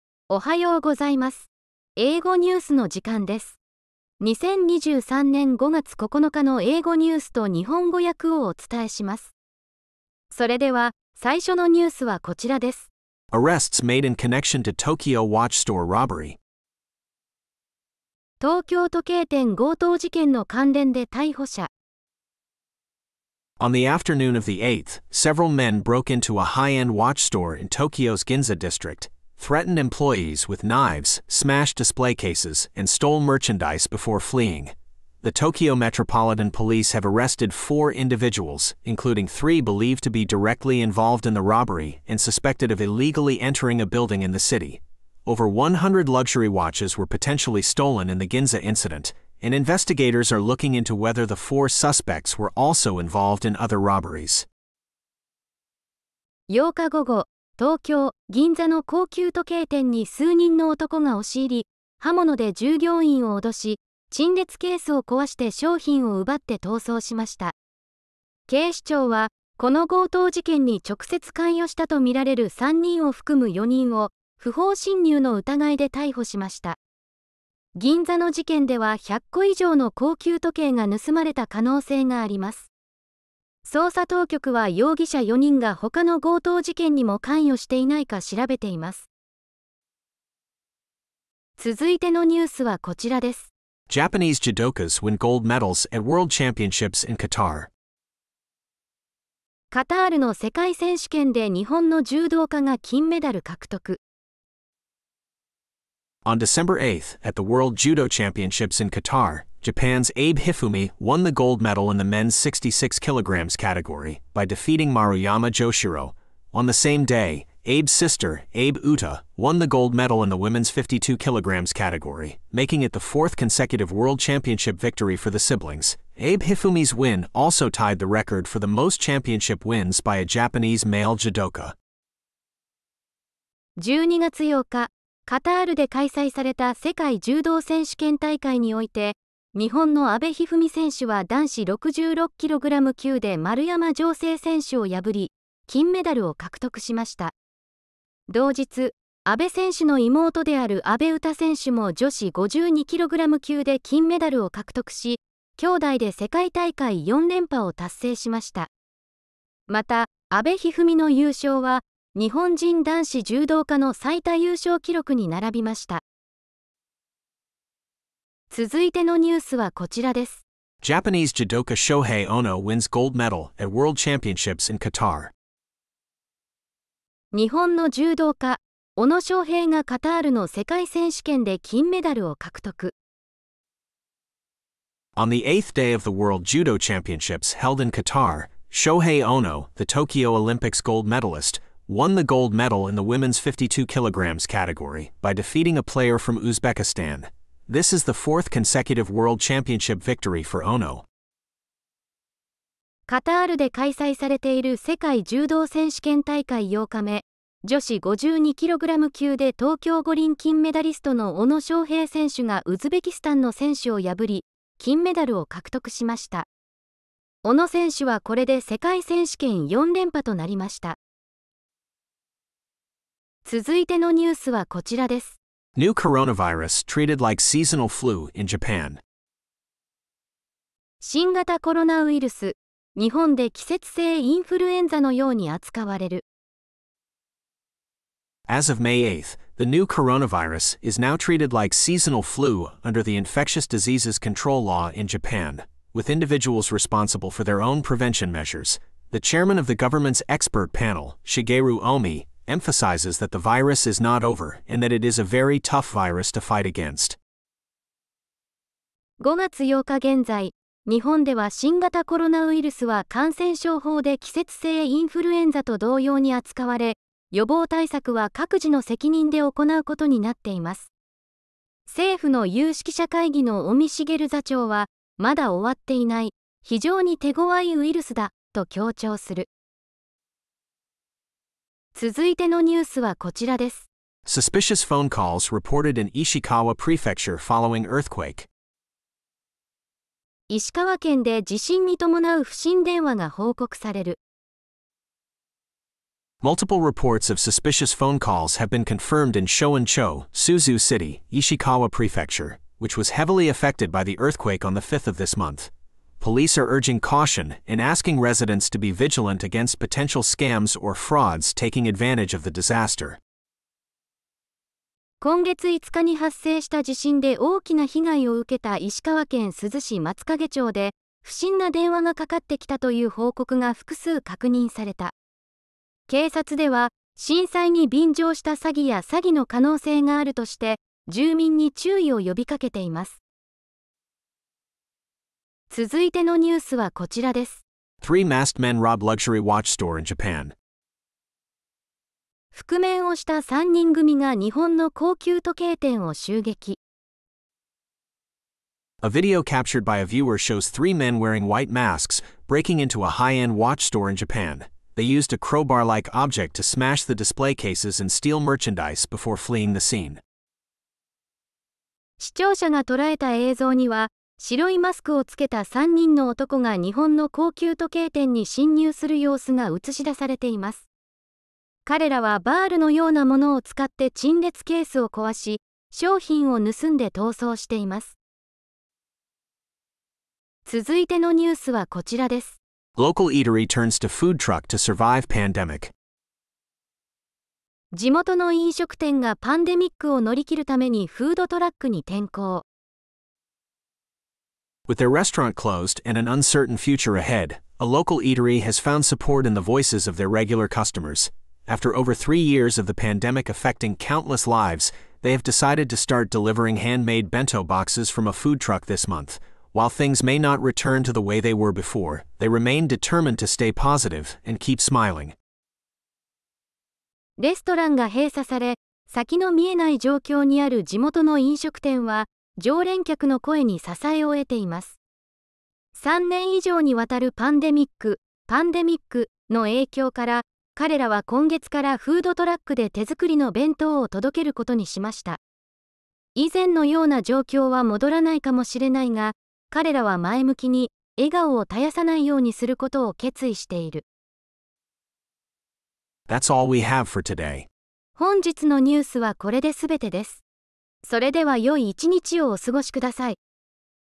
【日英対訳！毎日英語ニュース】5月9日「Arrests Made in Connection to Tokyo Watch Store Robbery」
毎日のニュースを読む時間に英語学習を取り入れませんか？本動画では、各ニュースについて、英語の音声が1回流れたあとに、日本語の音声が1回流れます。声を出せる方は、英語の音声をなぞるように、自分でも発音することで、ニュースで頻出する表現がいつでも自然に出てくるように脳に定着させることができますよ。日本語の音声では、そのフレーズの意味を確認するようにしてください。